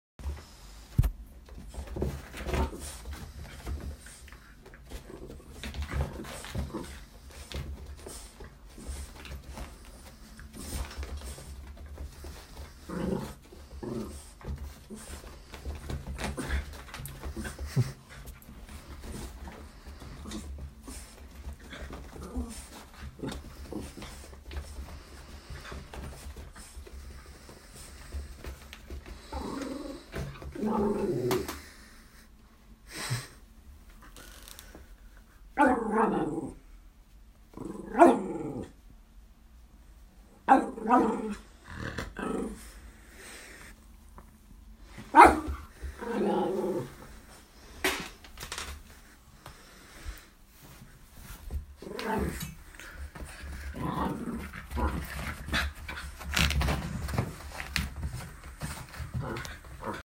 Siamese Kitten Playing with Dog
Our Siamese Kitten Simba playing with our Dog Wolfie.